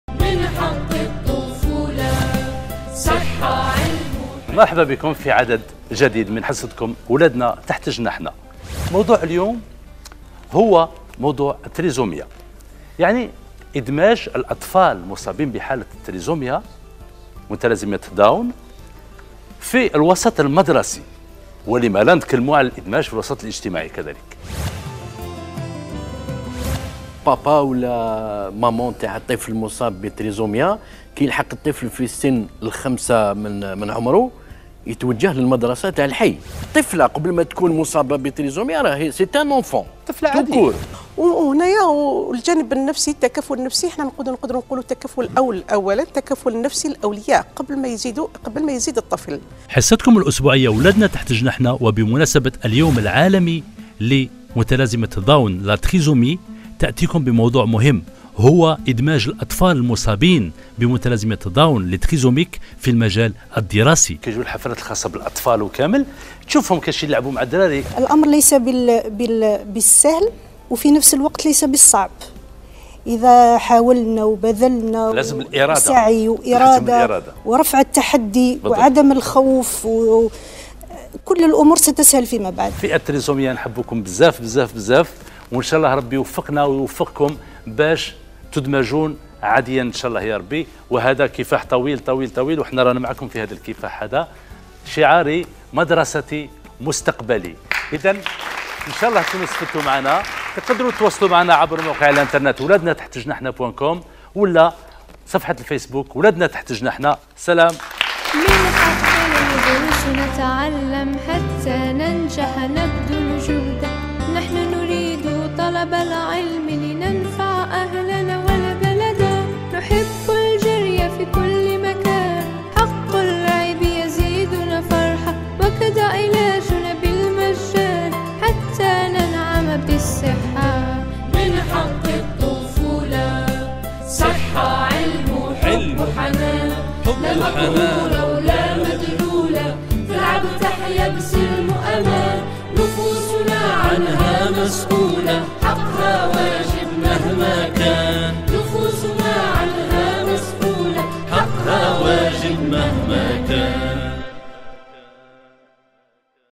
اعلان اولادنا تحت جناحنا باليوم العالمي للتريزوميا - Wladna Taht Jnahna Trisomie du 24 03 2018
اعلان اولادنا تحت جناحنا خاصة باليوم العالمي للتريزوميا ليوم 24 مارس 2018 - Wladna Taht Jnahna dédiée à la Trisomie du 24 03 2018